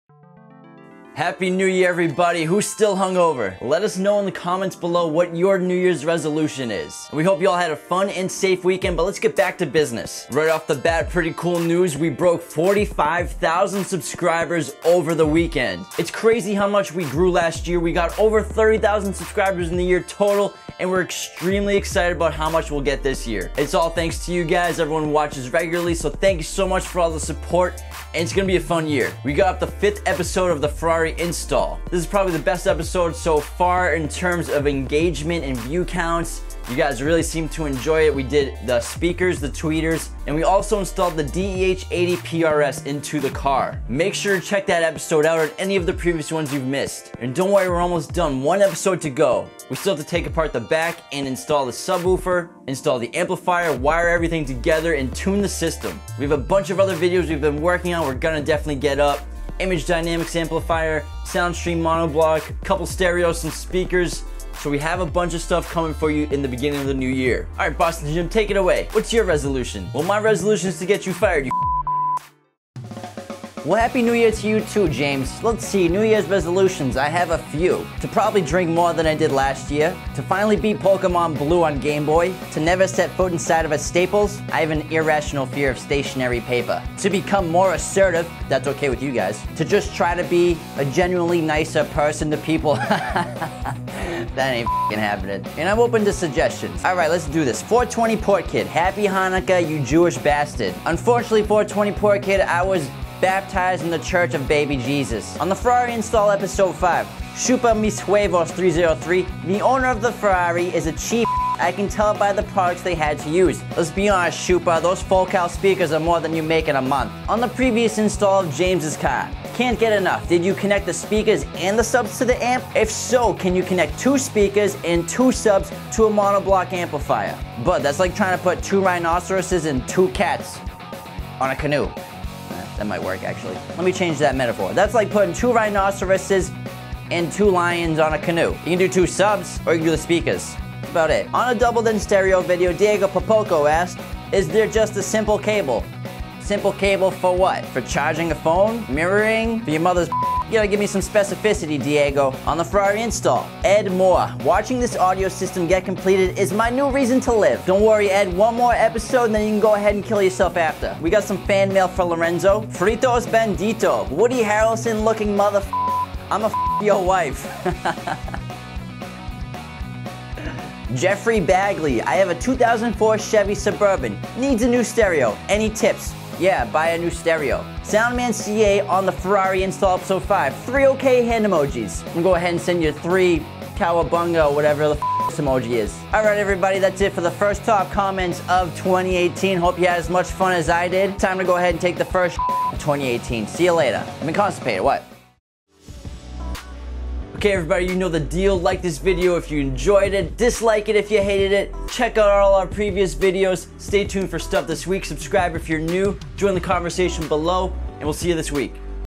QMV Vlog 75: One more Ferrari Episode left!
Only one more episode left of our Ferrari Audio System Installation!!!

for all of your car audio and video needs!